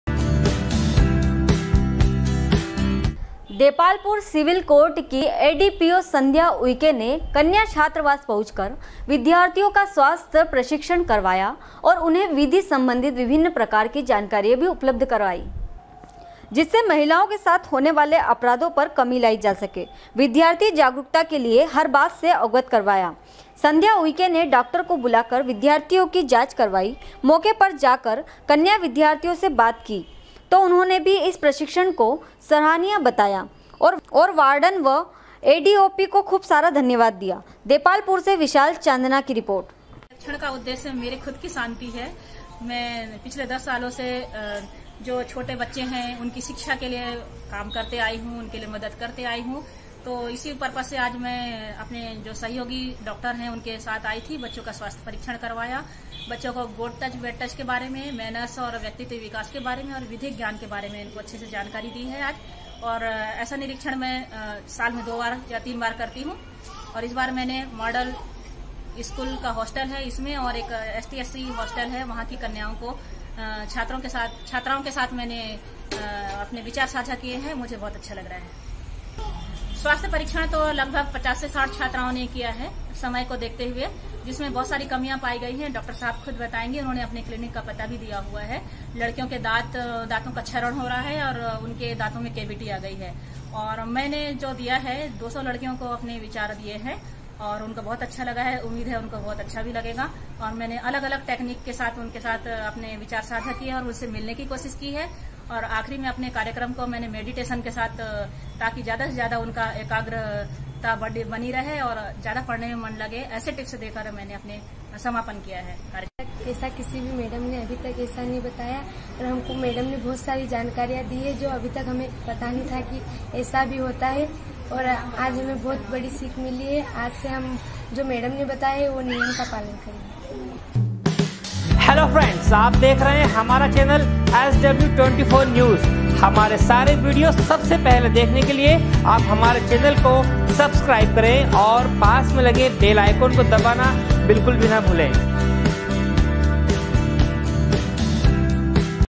sw24news depalpur, 03.09.2019
देपालपुर सिविल कोर्ट की एडीपीओ संध्या उईके ने कन्या छात्रावास पहुंचकर विद्यार्थियों का स्वास्थ्य प्रशिक्षण करवाया और उन्हें विधि संबंधित विभिन्न प्रकार की जानकारी भी उपलब्ध करवाई#sw24news